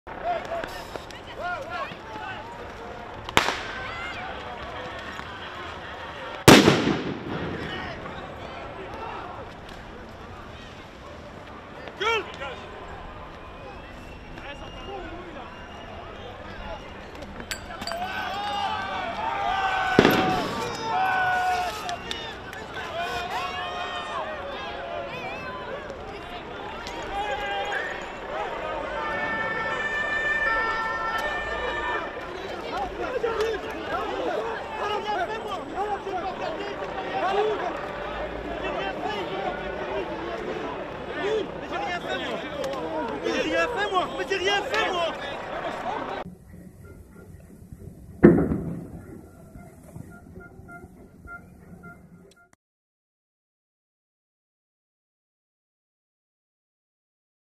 Belgian Protest Leaves Police Chief Concussed
Brussels Police Chief Pierre Vandersmissen was injured in a clash with protesters in the Belgian capital on Tuesday after he was hit  in the head by a demonstrator.